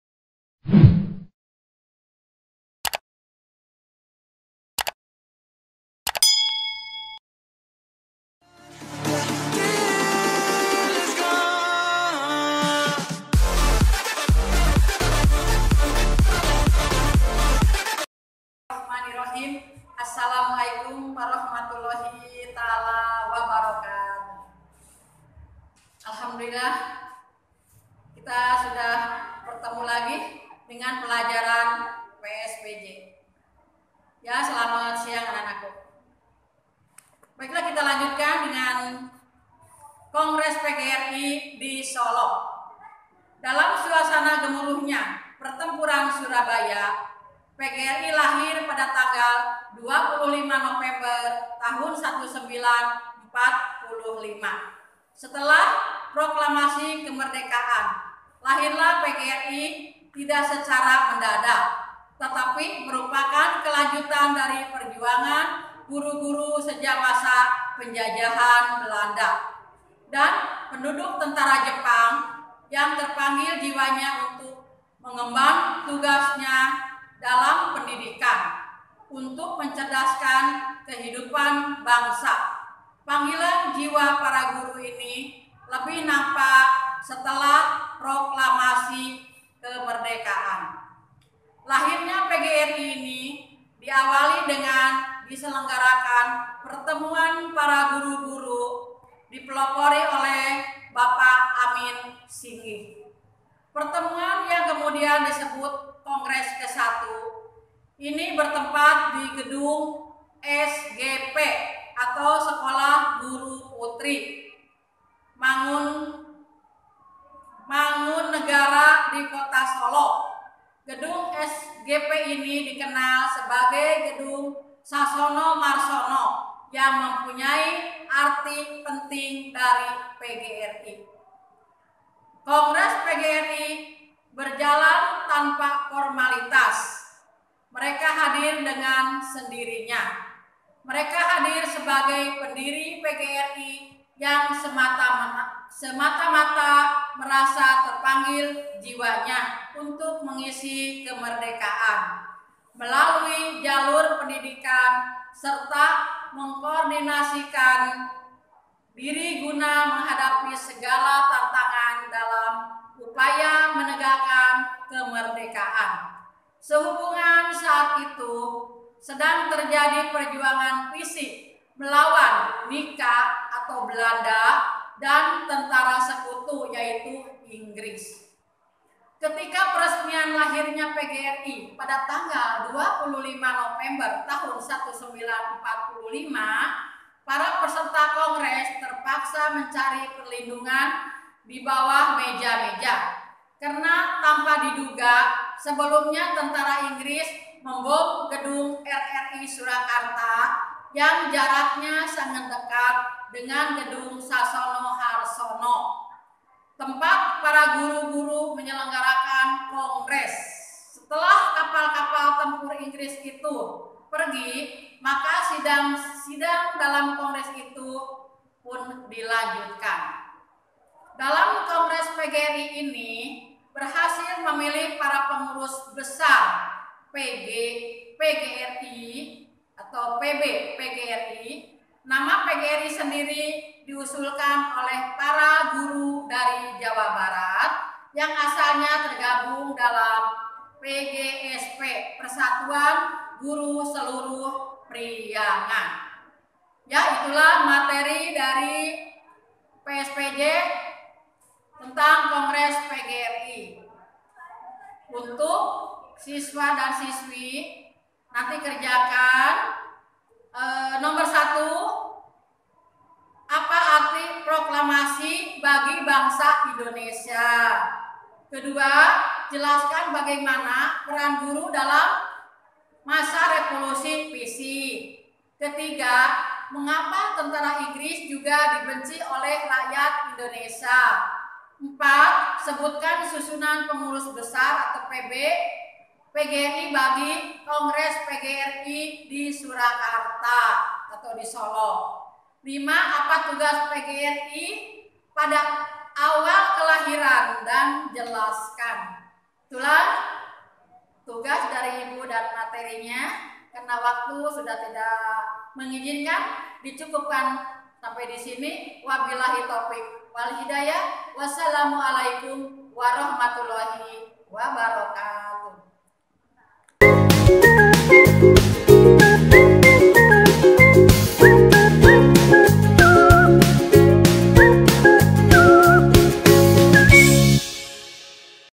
XII PSPJ PERTEMUAN KE 7
Pembelajaran Jarak Jauh Mata Pelajaran pspj Kelas XII pertemuan ke-7

Dalam Pandemi ini SMK PGRI 2 KARAWANG, membuat salah satu Metode Pembelajaran Jarak Jauh yaitu dengan Video Pembelajaran yang memanfaatkan Youtube